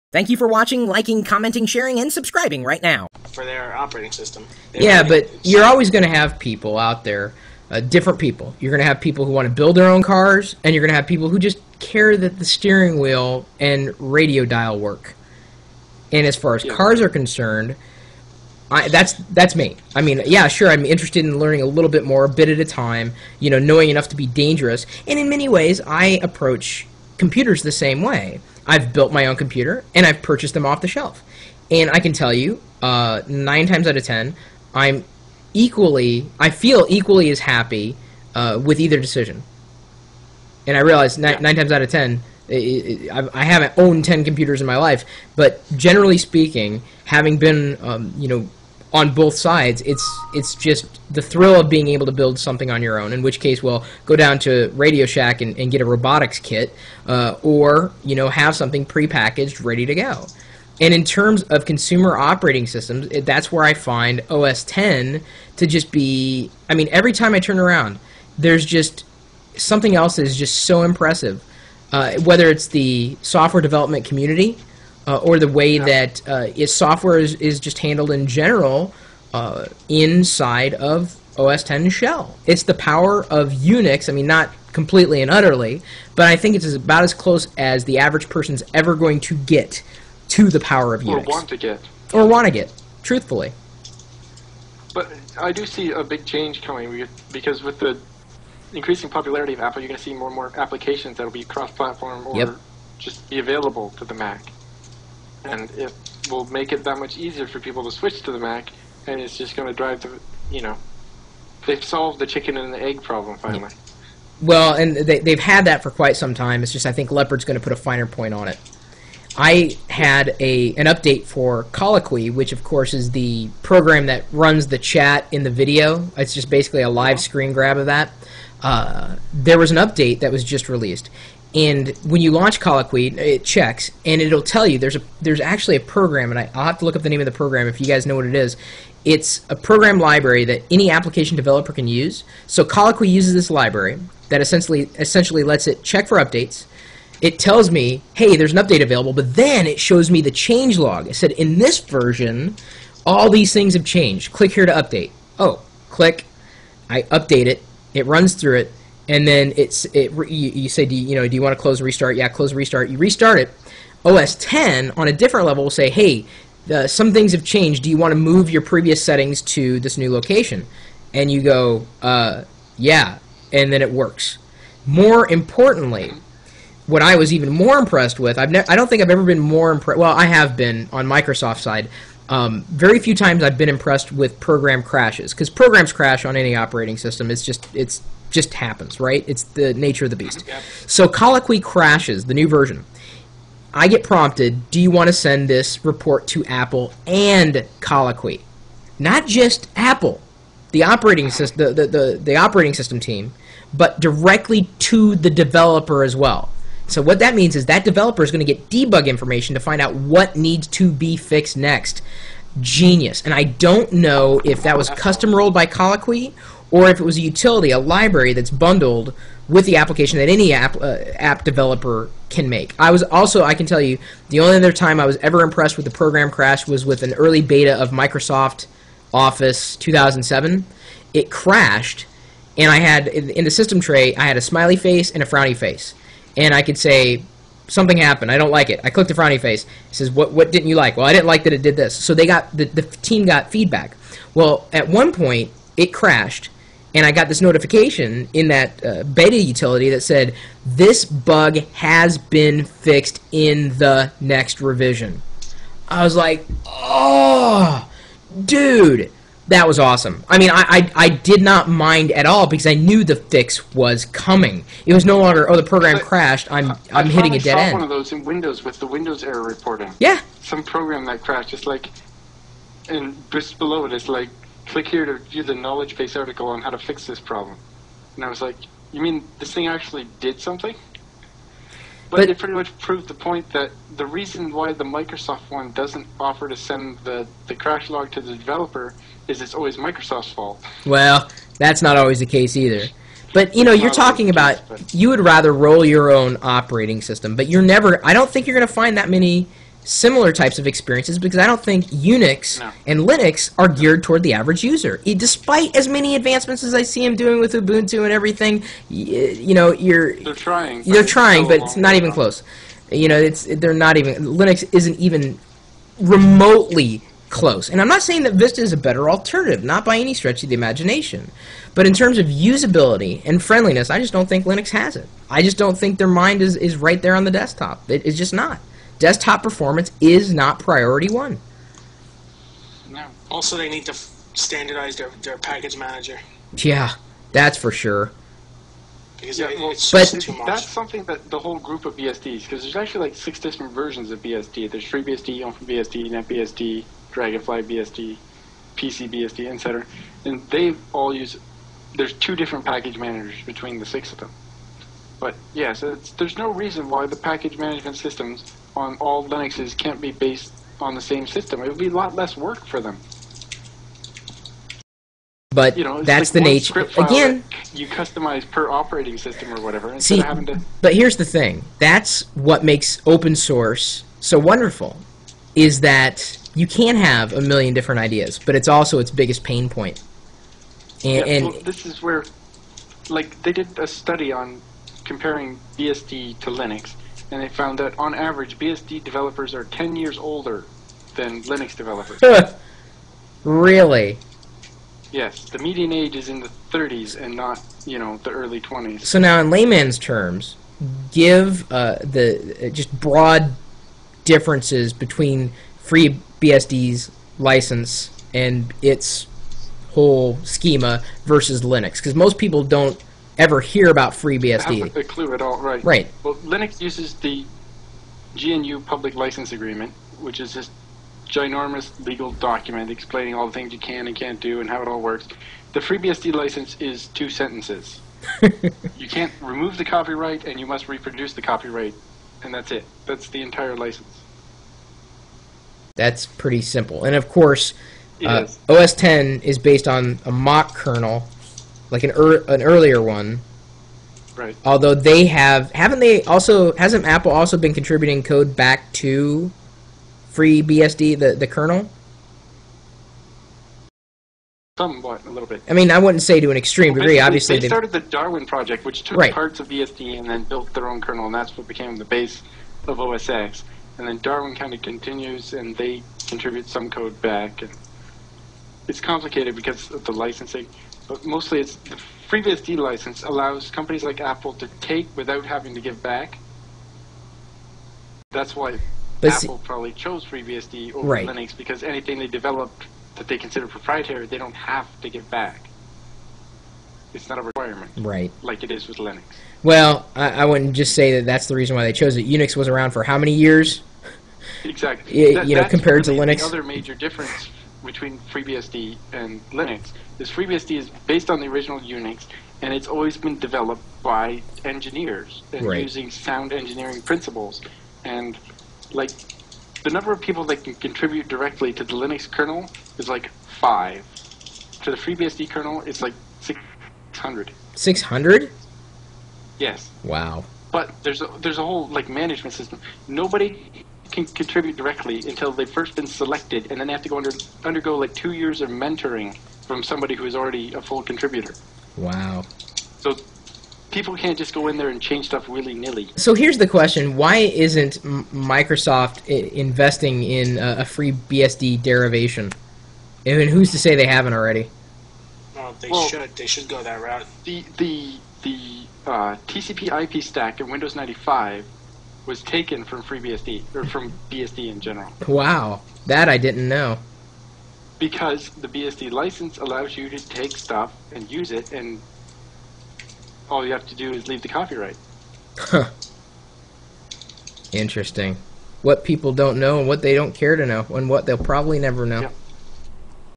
Software Choices, Crashes, and Updates
- Two of my regular community members joined me via Skype for this call. The three of us tossed around opinions and ideas regarding software... your choices, differences, crashes, and even updating.